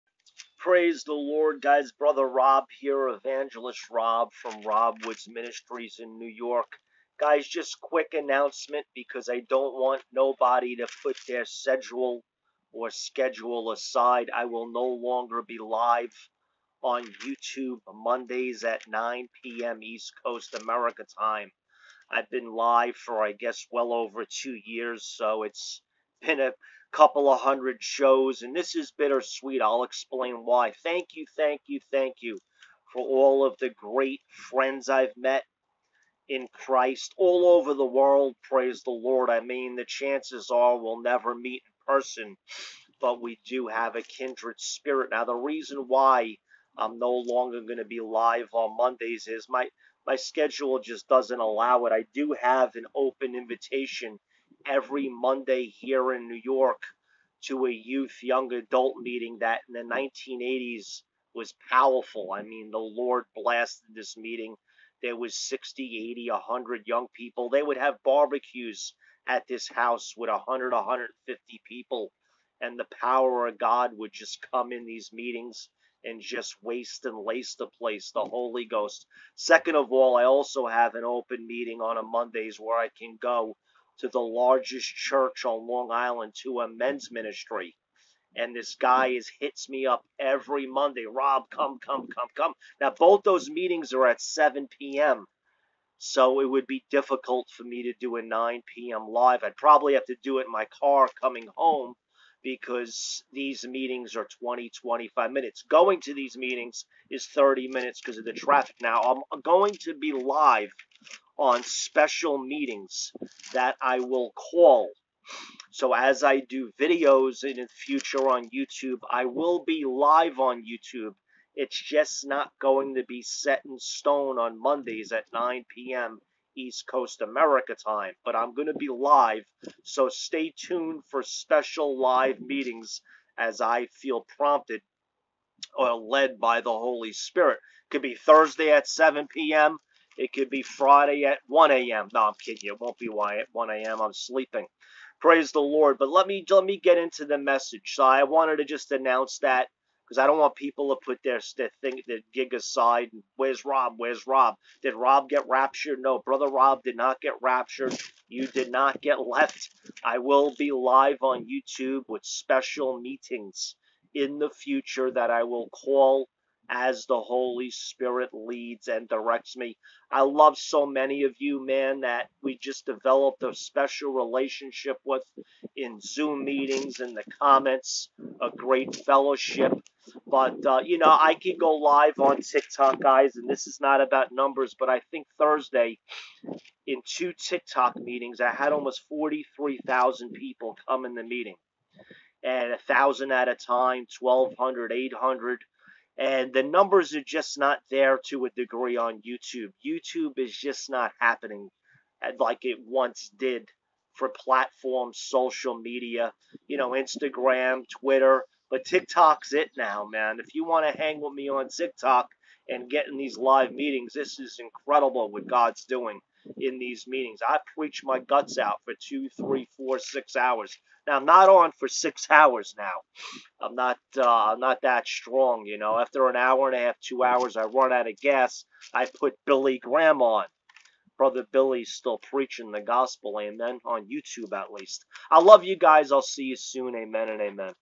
Prophetic word the LORD SAYS SPECIAL ANNOUNCEMENT FROM BROTHER ROBB TO ALL!!!
my friends I will only be calling special live meetings on YouTube in the future as I will no longer be live every Monday